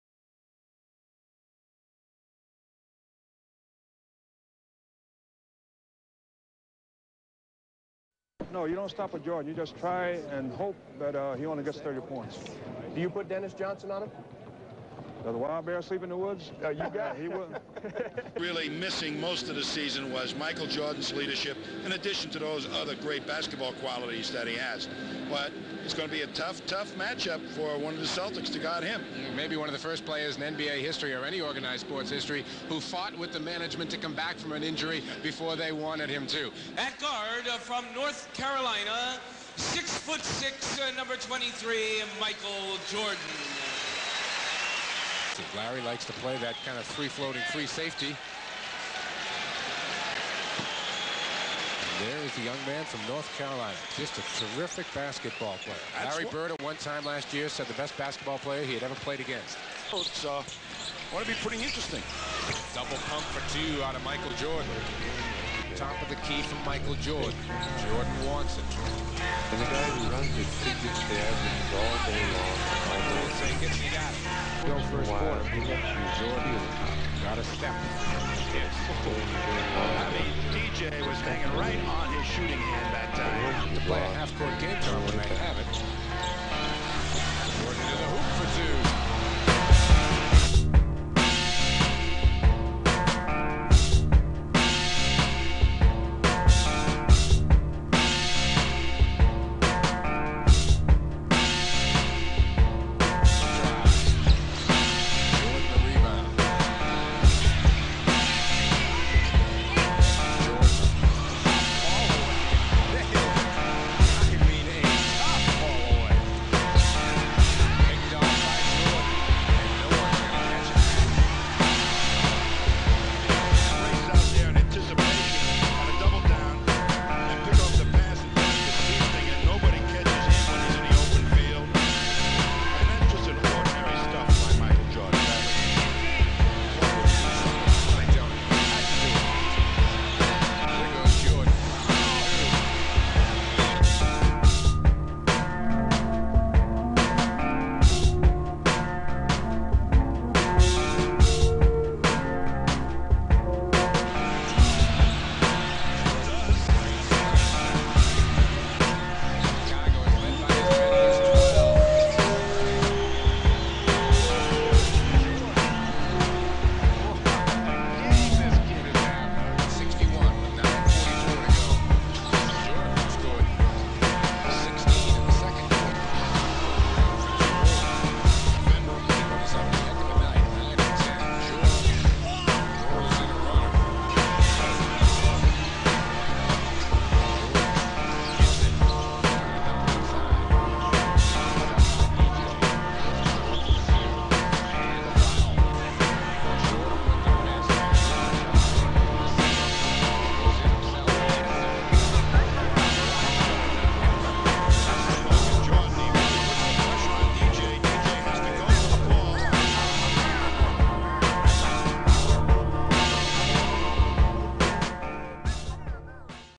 Michael Jordan 1986 Playoffs: Gm 1 Vs. Celtics, 49 Pts
History tends to forget this game merely because in Game 2, MJ scored 63. But this game had MJ attacking the basket more since in Game 2, the Celtics tried to close the driving lanes. It didn't matter, MJ dominated with an inside or outside game even after a 64 game layoff with a broken foot. After this game, Bird said they'd never let him scored 49 again. He was right - he got 63 next. These were the Celtics that won the Championship that year. MJ showed the world what he was capable of against the greatest team in just his 2nd year. Young MJ filled up the highlight reel every game.